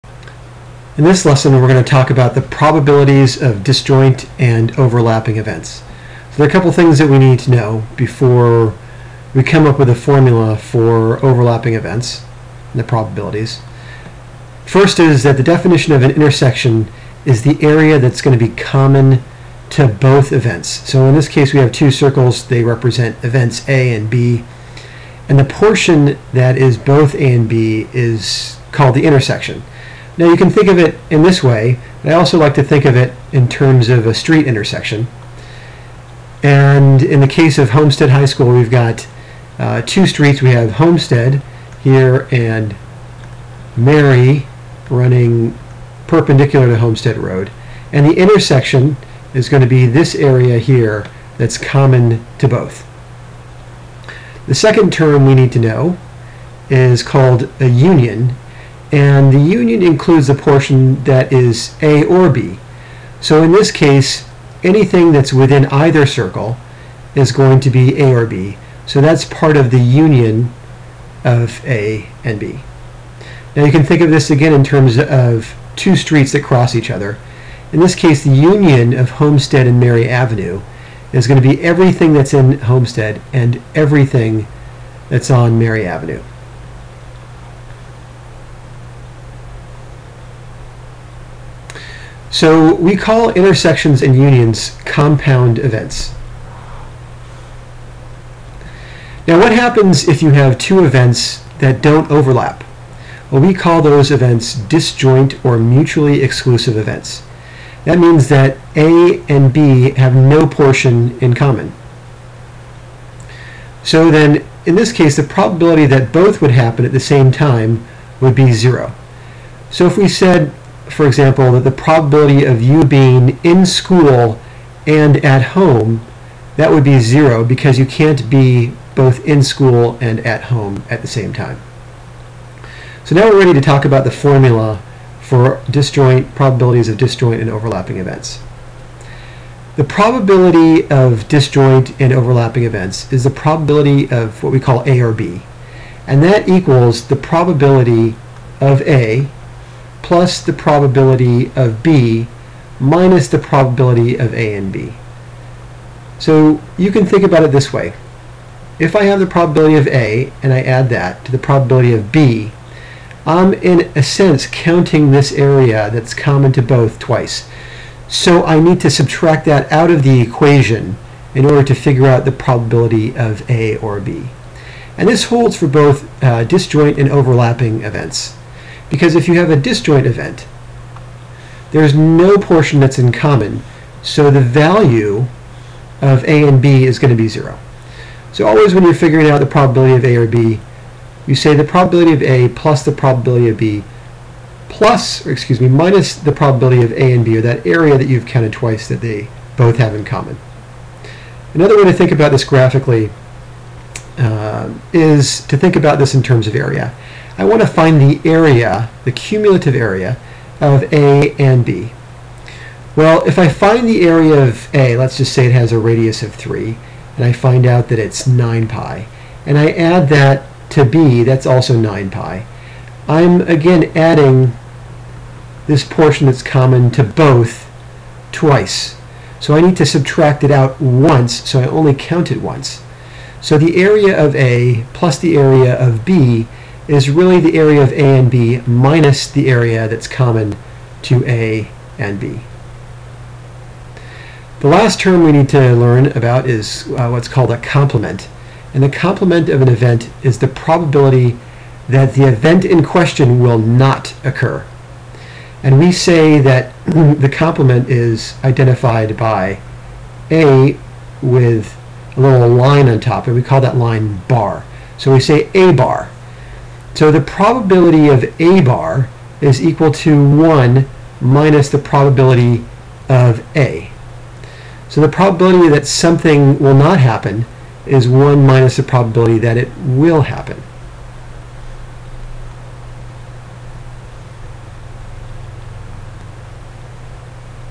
10.4 Probabilities of Disjoint and Overlapping Events (Lesson)
A lesson on probabilities of overlapping and disjoint events with explanations of formulas to determine probabilities